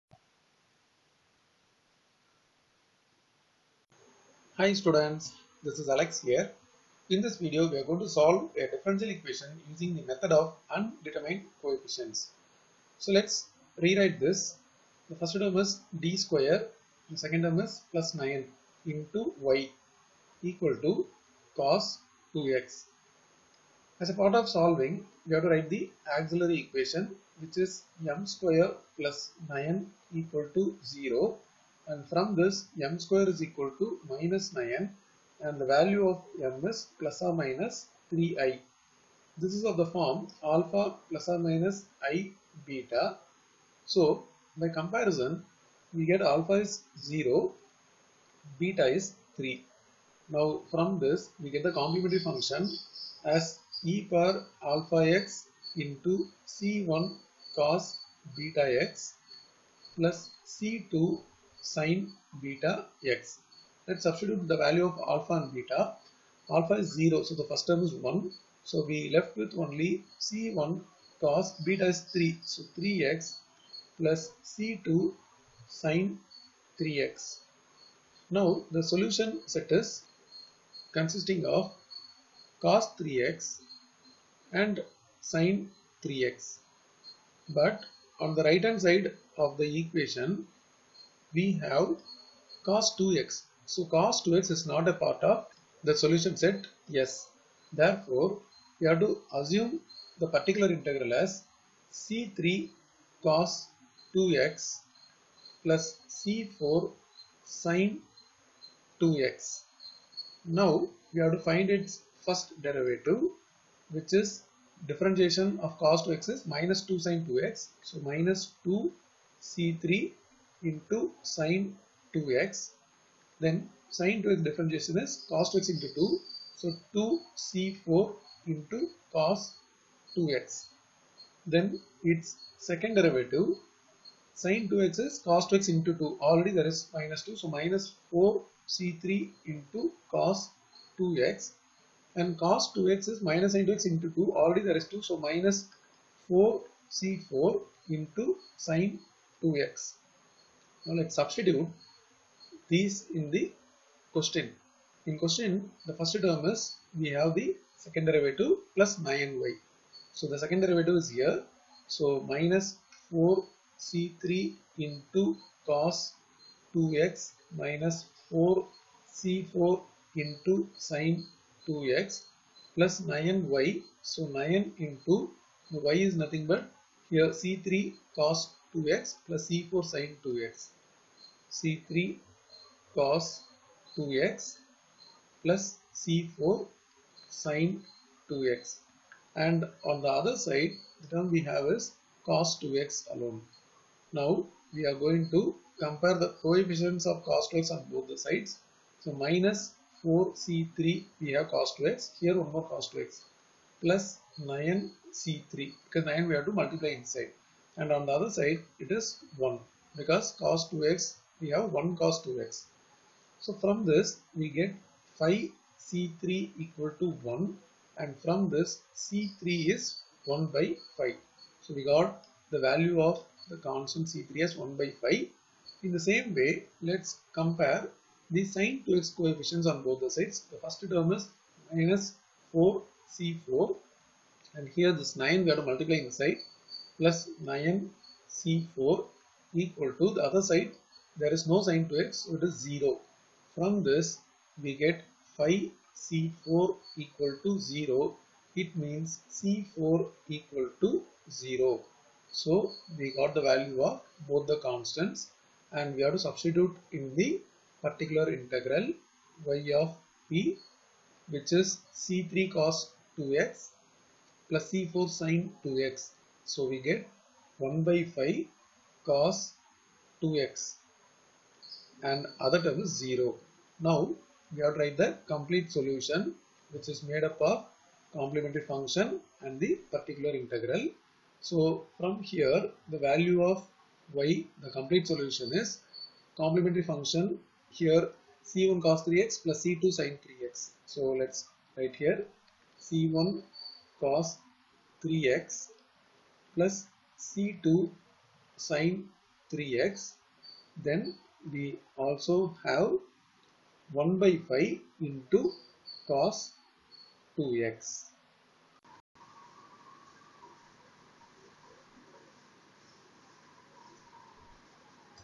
Method of undetermined coefficients Problem 3 Engineering Mathematics M1 Differential equations
Higher order linear differential equations with constant coefficients – Method of variation of parameters. Homogenous equation of Euler’s and Legendre’s type – System of simultaneous linear differential equations with constant coefficients – Method of undetermined coefficients
method of undetermined coefficients to find a particular solution,